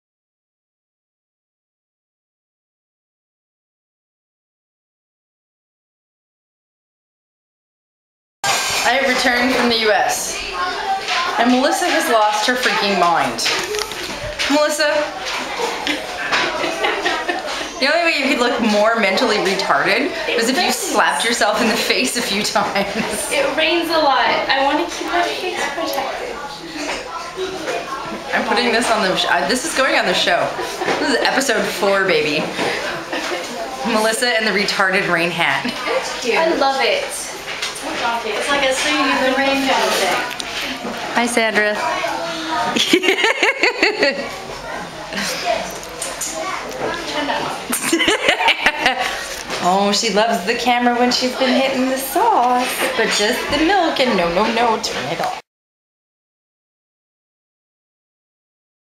M&M Ep 4: Little rain hat, or retard suit? ..... you decide.
Episode 4: Little Yellow Hat

I got back and Mel was sporting this fashion wonder.. Christ I can't leave that girl alone for a minute...

Go ahead and leave your little comments on how cute Mel is and how mean I am ... :P  ( I don't care, I still think she looks like a 'tard monkey)